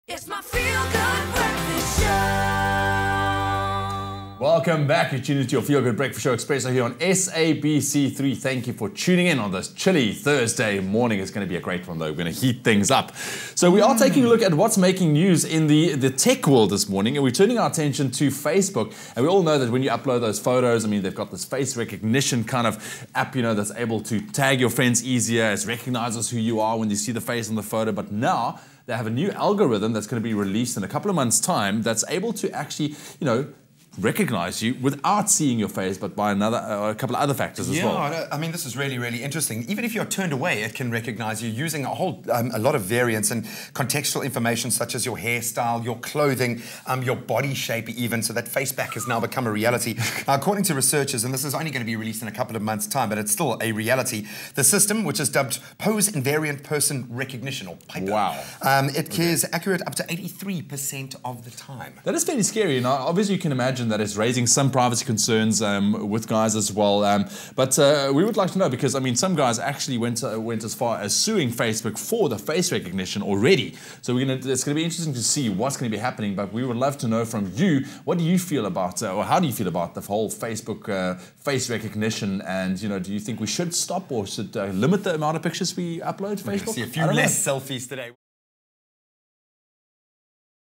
Facebook to launch Facial Recognition Tech
Facebook researchers have unveiled new research that allows them to recognise you without even needing to see your face.